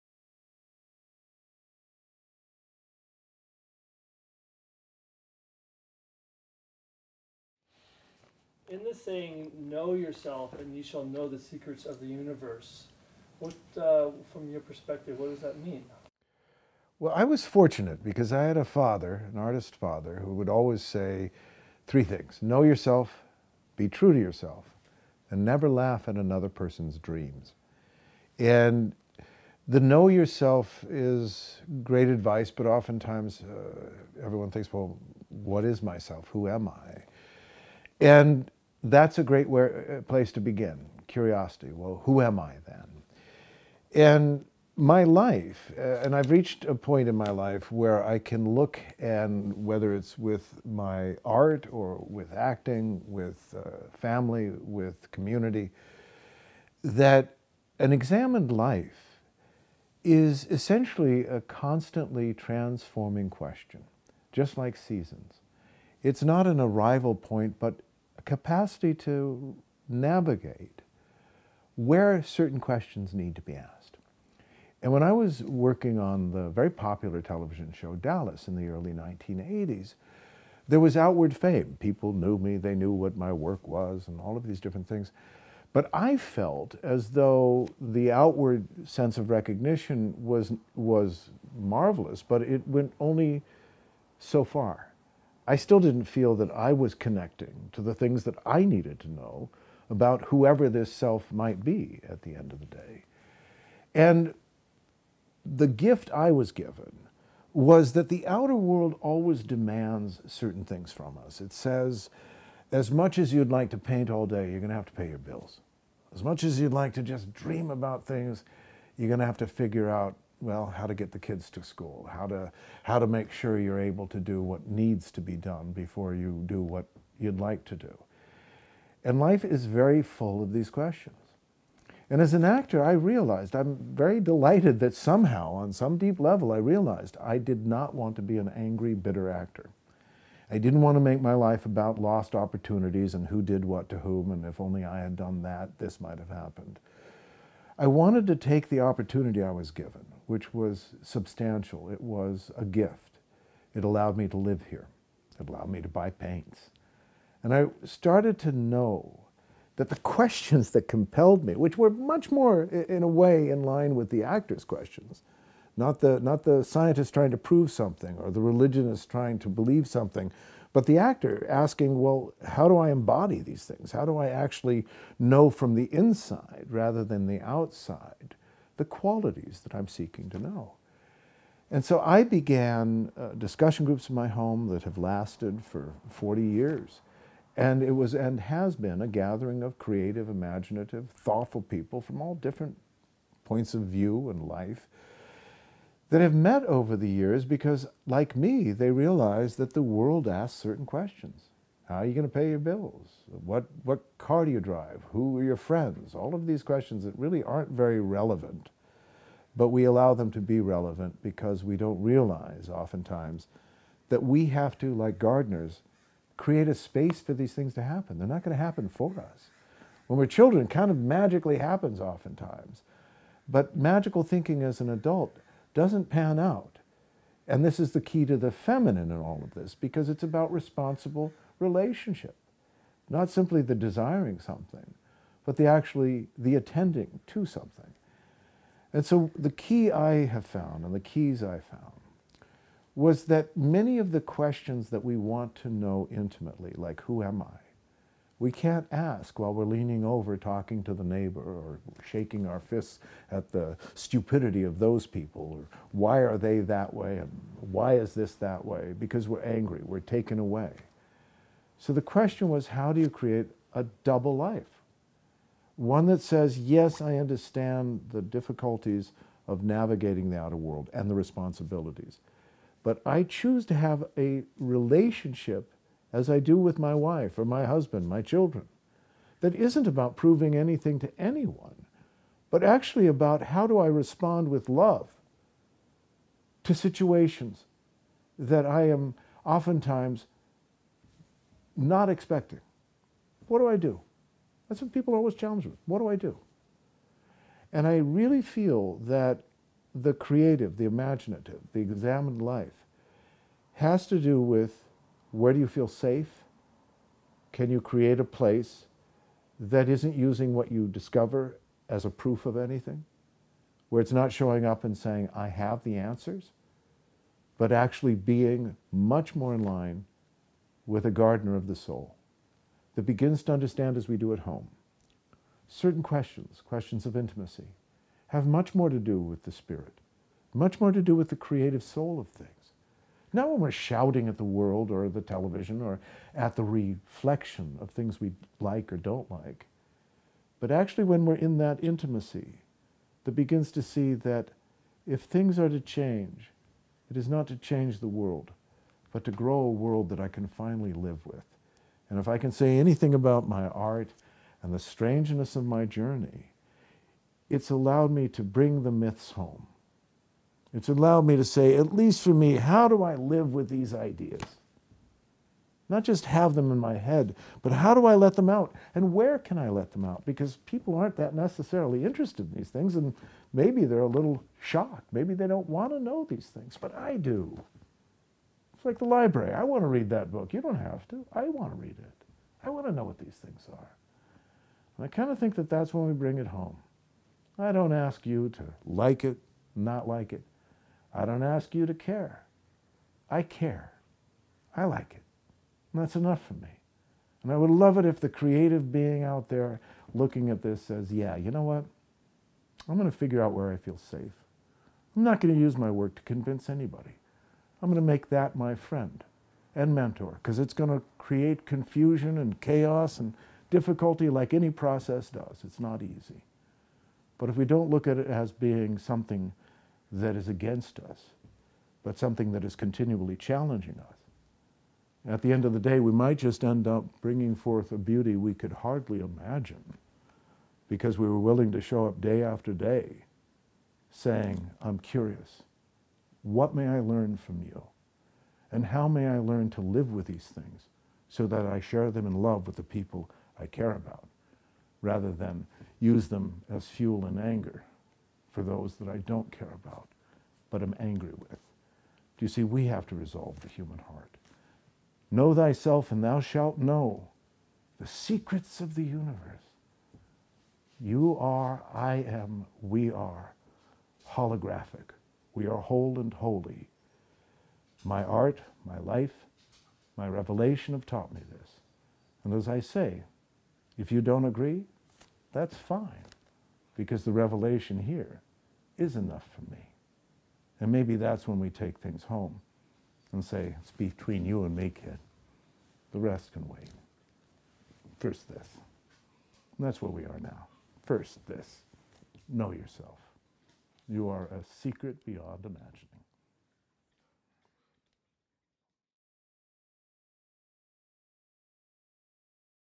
KNOW THYSELF with Leigh J McCloskey
A deep and Intimate look at living an examined life in an often times difficult and angry world. “An examined life is essentially a constantly transforming question. Just like seasons, it’s not an arrival point, but a capacity to navigate where certain questions need to be asked.”